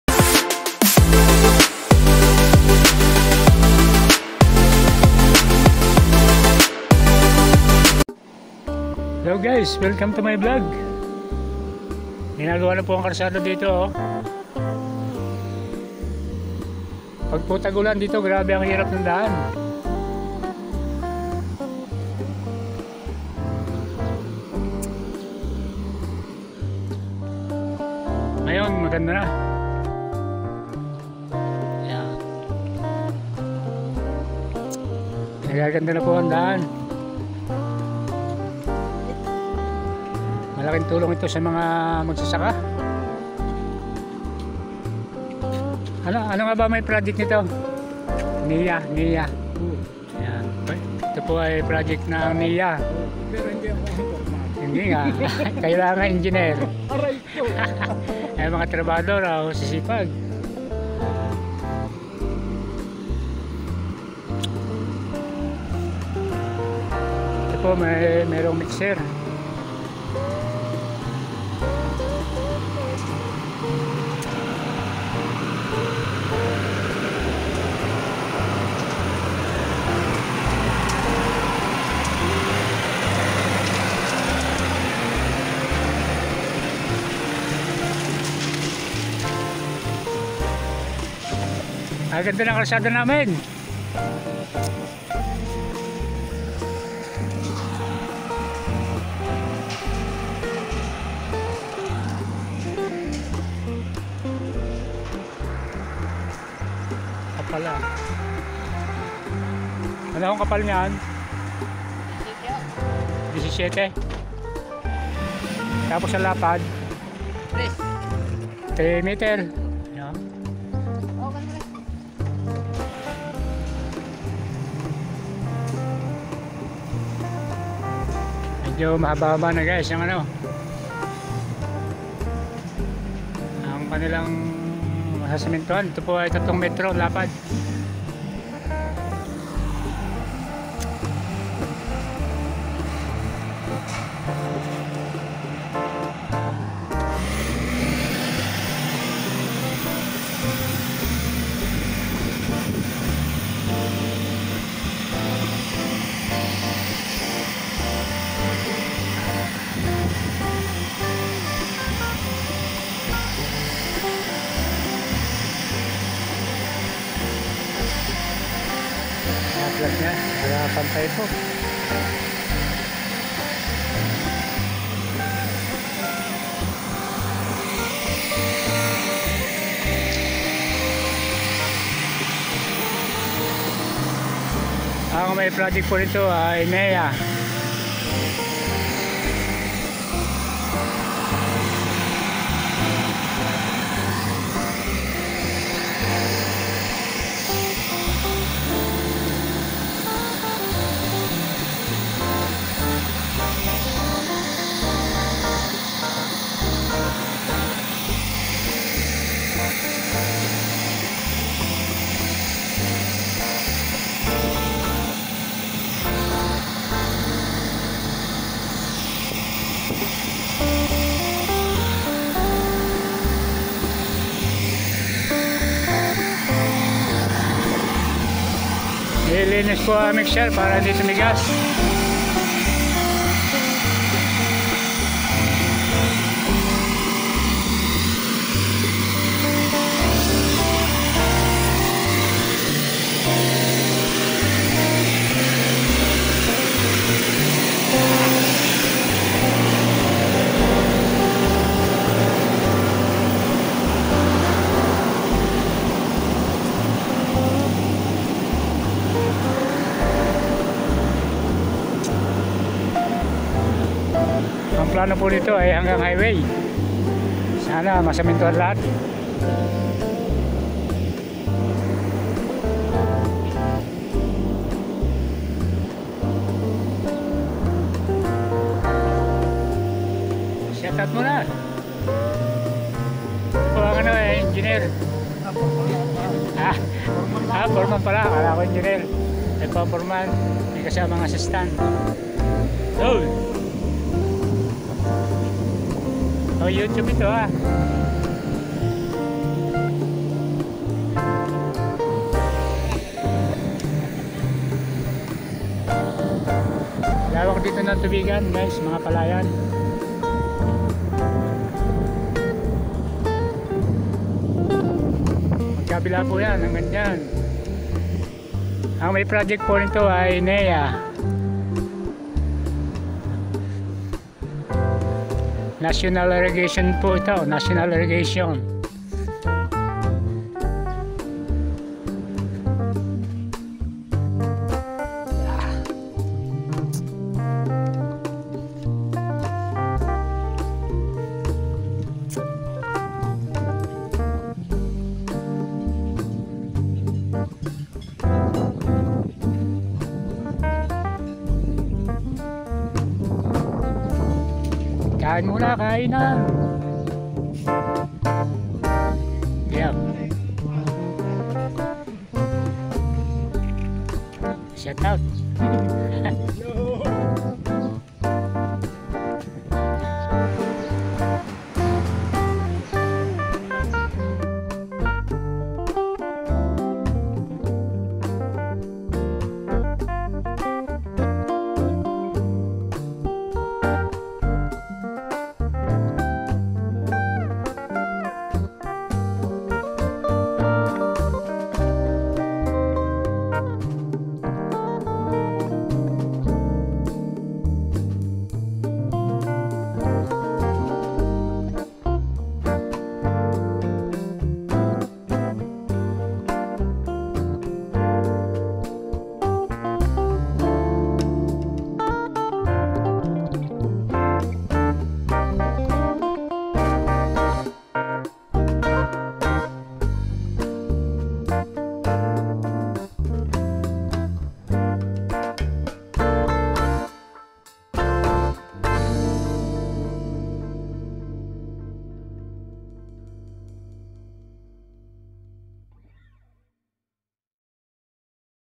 KALSADA PINAPAGANDA,way farmers barangay amazing ,@batangenyongmagsasakatv5632
THIS PROJECT IS  COVERED BY NATIONAL IRREGATIO ADMINISTRATION,gaganda Ang daan Ng mga magsasaka#farmers